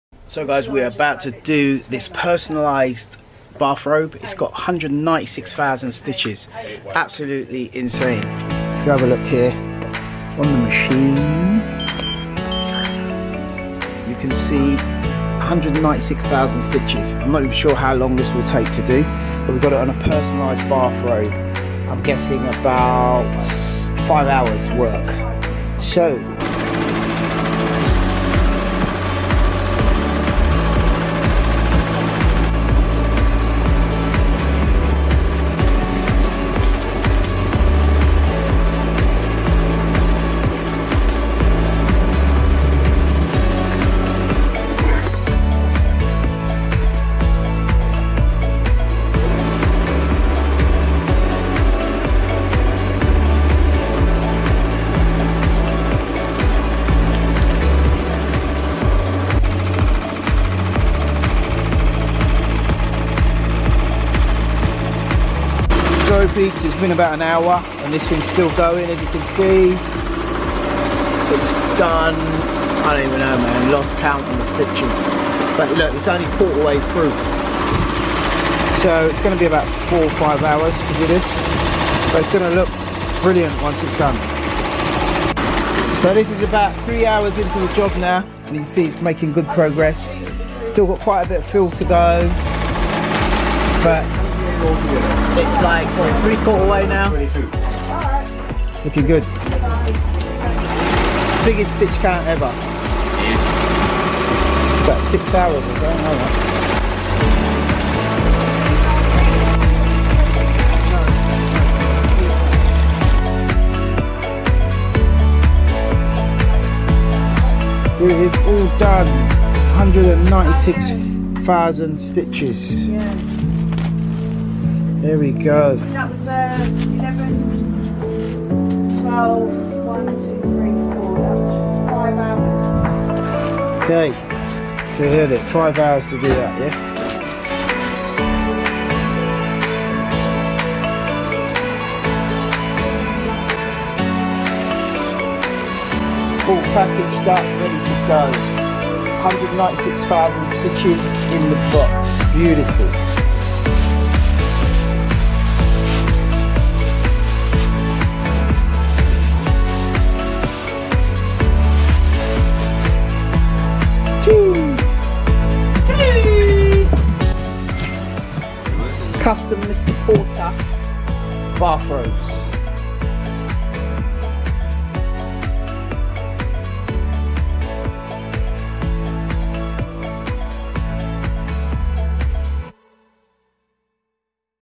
Custom Embroidery MR Porter Bathrobe! ( 196k stitches! )
196k stitches! on a bathrobe | Our biggest Custom Embroidery stitch count yet